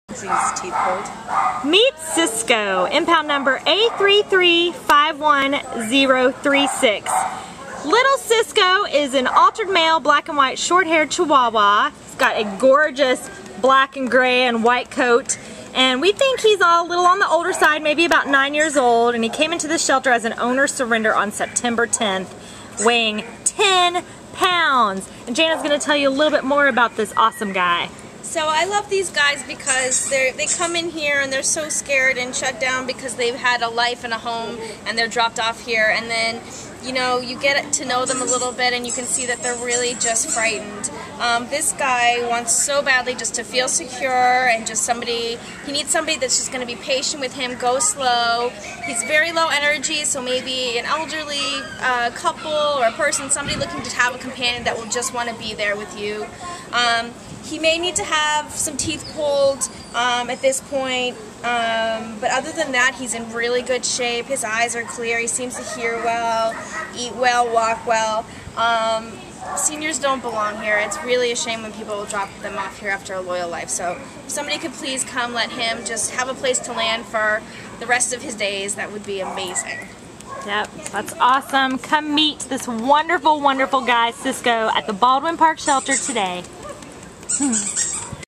Cisco A3351036 R.I.P.
Cisco was a spry nine year old black and white neutered male Short Haired Chihuahua mix who was dumped at the Baldwin Park Shelter by his former owner on September 10th.  Weighing ten pounds, Cisco was a low energy dog who liked to be held.  He was at his best with a patient handler who made him feel secure, and he wanted nothing more than a lap to call his own.  Cisco would have been the perfect indoor pet for an adult household in any living situation and an the ideal companion for a senior but no one cared enough to adopt him and on September 26th he was led to the back of the shelter and given a lethal Marcia Mayeda cocktail.  His carcass was placed inside a barrel of dead dogs inside a freezer and now awaits shipment to a rendering plant where it will be boiled and the fat drained to produce lipstick and other cosmetics.  Cisco is at least the 580,542th victim of Marcia Mayeda's reign of error as head of the Los Angeles Department of Animal 'Care' and Control.